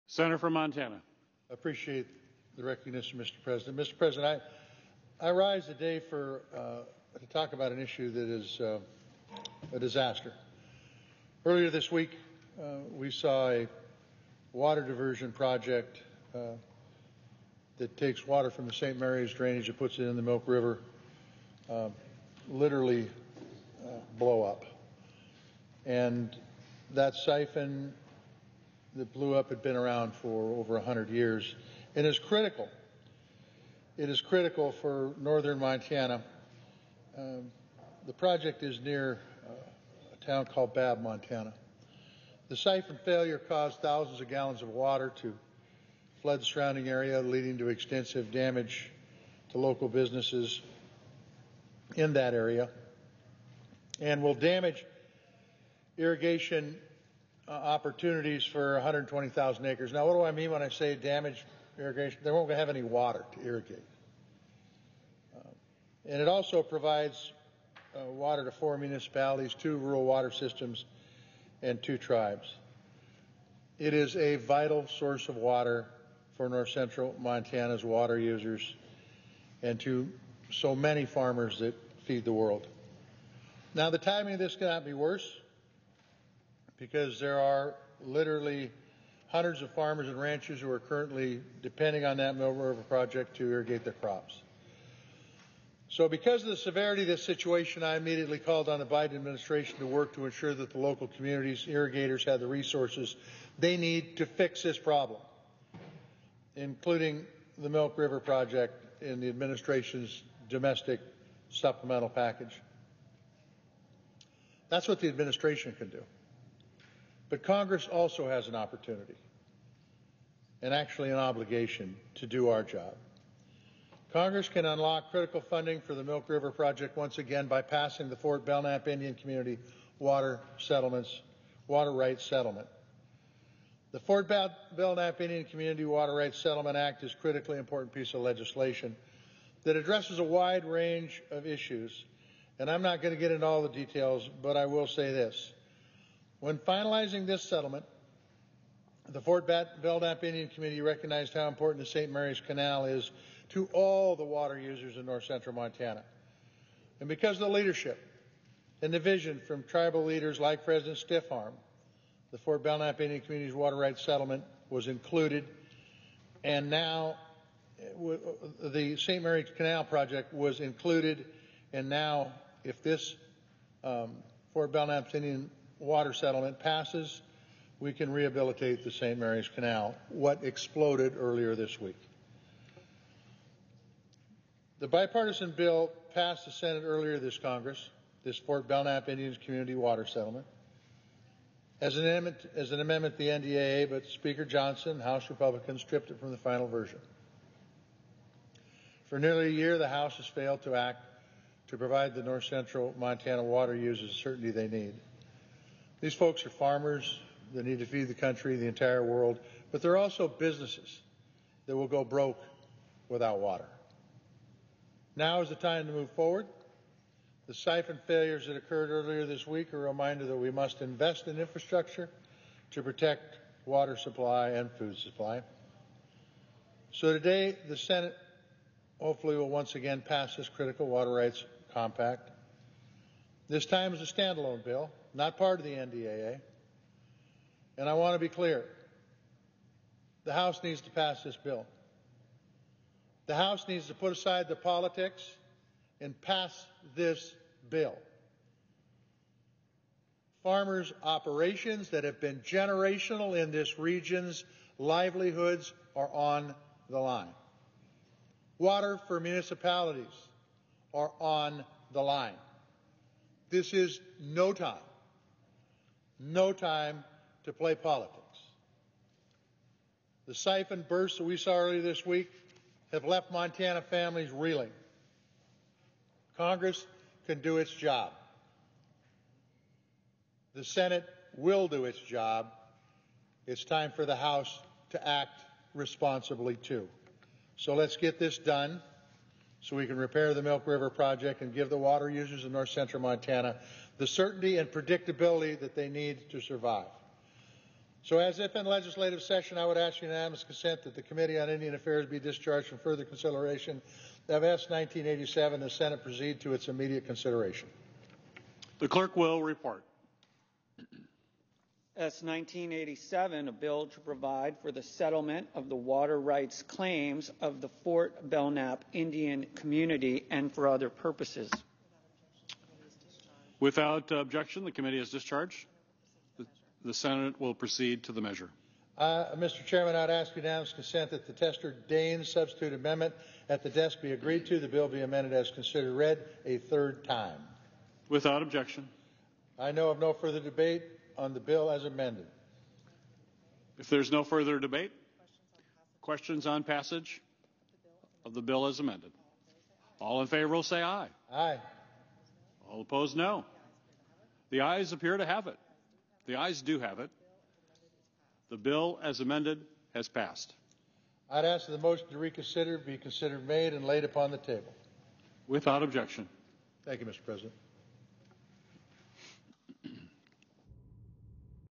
S.1987, the Fort Belknap Indian Community Water Rights Settlement Act
The U.S. Senate considers S.1987, the Fort Belknap Indian Community Water Rights Settlement Act, on June 20, 2024.

The bill ratifies a water rights settlement for the Fort Belknap Indian Community, headquartered in Montana. It is the last remaining tribal water settlement in the state.

The bill was approved by unanimous consent. It can now be considered by the U.S. House of Representatives for further action.